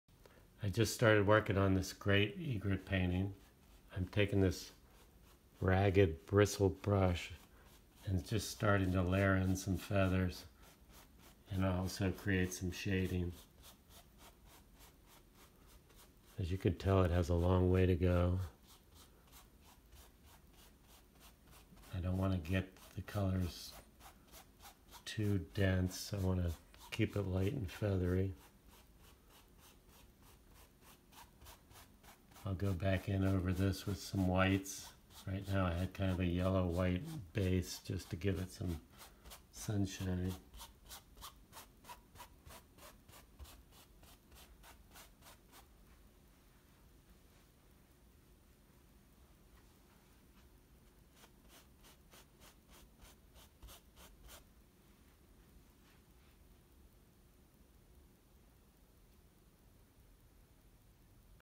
LINE OF SIGHT Work in progress video
Here I am working away on my latest painting of a Great White Egret.  Just getting started as you can see. This is a large bird portrait, 30 x 30 on canvas.

You can view all of my work on my and you can see the finished painting